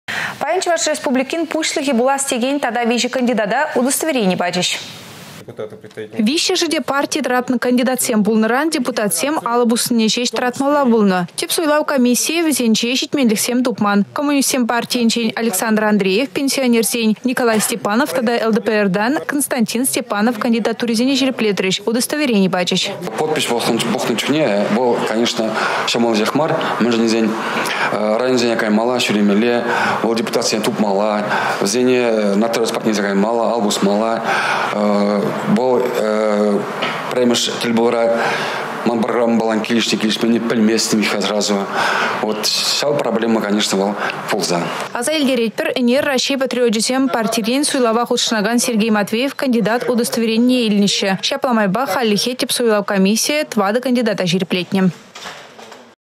Кандидатсене - удостоверени
Паян Чăваш Республикин Пуçлăхĕ пулас текен тата виçĕ кандидата удостоверени пачĕç.